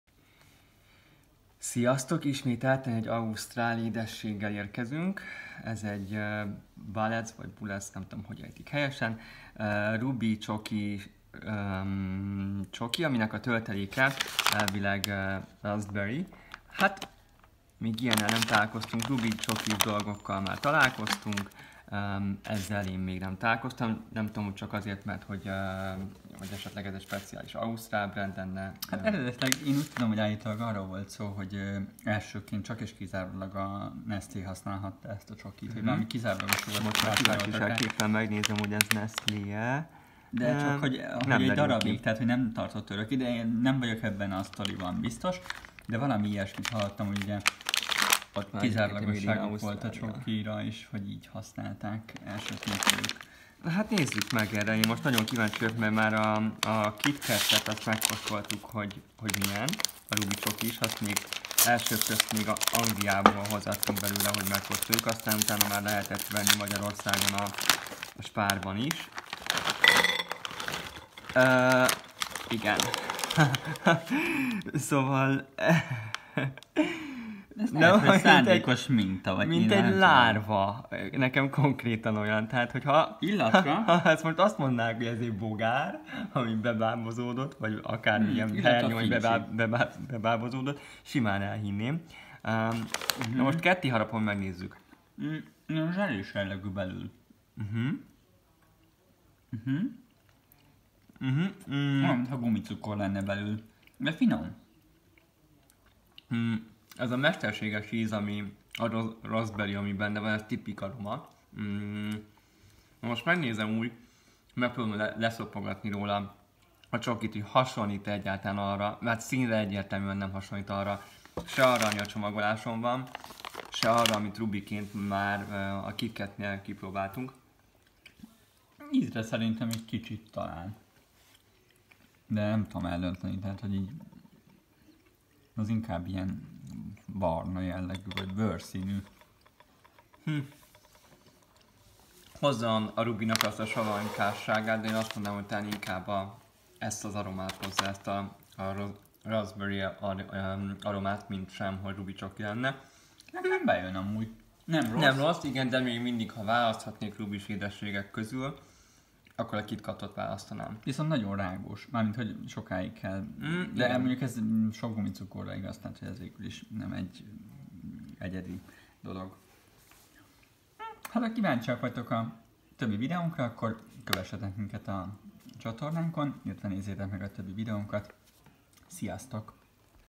Ausztrál Ruby csokit véleményeztünk!
Megnéztük milyen az ausztrál Ruby csokis nasi.